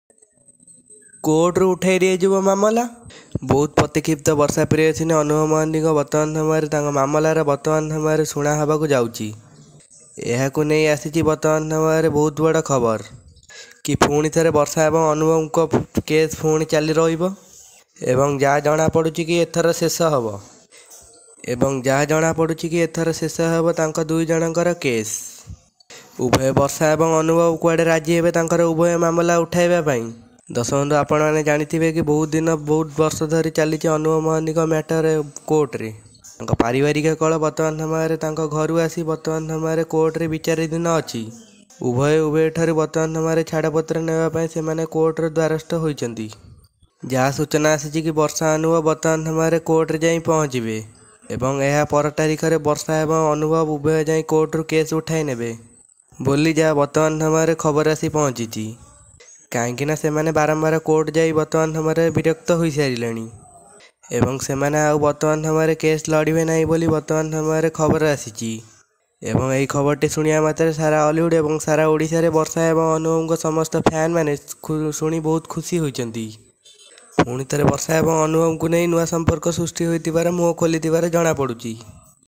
କୋର୍ଟରୁ ମାମଲା ଉଠିଲା,ଏକାଠି ହେଲେ ବର୍ଷା ଅନୁଭବ ll Barsha Priyadarshini and Anubhav mohanty ll
କୋର୍ଟରୁ ମାମଲା ଉଠିଲା,ଏକାଠି ହେଲେ ବର୍ଷା ଅନୁଭବ ll Barsha Priyadarshini and Anubhav mohanty ll Creative news